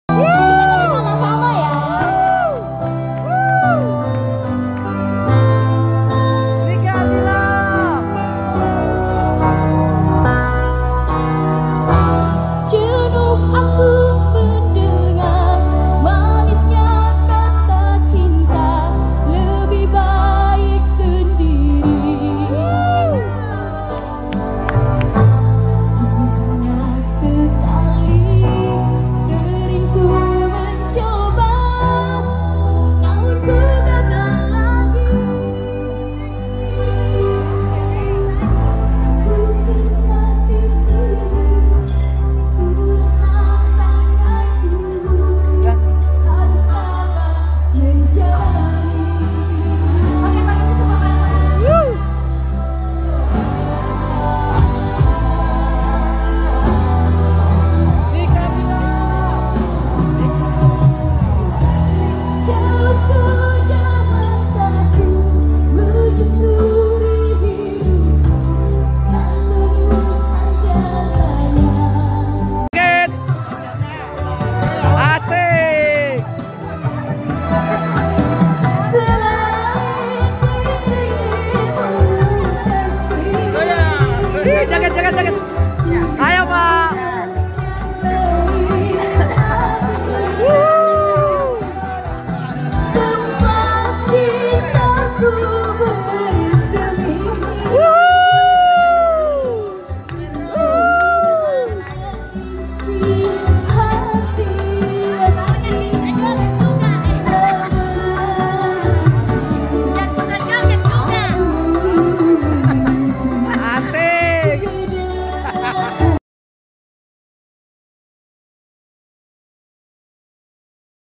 Rindu Suara Ressa 'Ardilla', Kini Kembali Bikin Heboh Acara Pangantin di Makassar
Resa, perempuan asal Jeneponto yang memiliki suara mirip dengan mendiang Nike Ardilla, baru saja tampil di Makassar. Ia menyanyi di sebuah pesta di Hotel Claro pada 14 Oktober 2024.